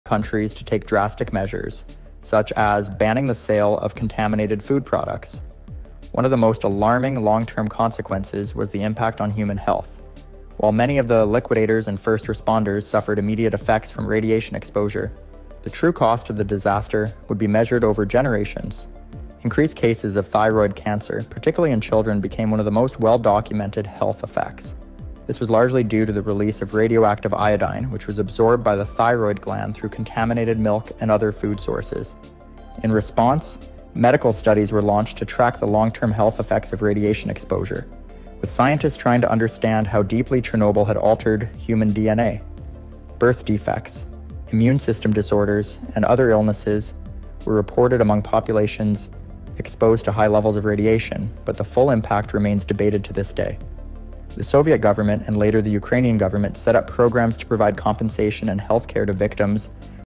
The Most Dangerous Nuclear Disaster In Human History
Just How Catastrophic Was The Chernobyl Disaster? Disaster Awareness Shorts Part 14

The Chernobyl disaster is one of the most devastating environmental catastrophes in human history, and yet, many of us still don't know the full extent of the horrific truth behind this Ukrainian disaster. From radiation exposure to nuclear safety, the consequences of this event are still felt today. In this video, we'll delve into the shocking facts and myths surrounding the Chernobyl disaster, exploring the historic events that led up to the catastrophe and the long-lasting impact it's had on the natural world.

We'll also examine the legends of the "Chernobyl ghosts" and the eerie remnants of the abandoned city. If you're interested in natural sciences and historic disasters, this video is for you. So, join us as we uncover the truth behind one of the most infamous nuclear disasters in history.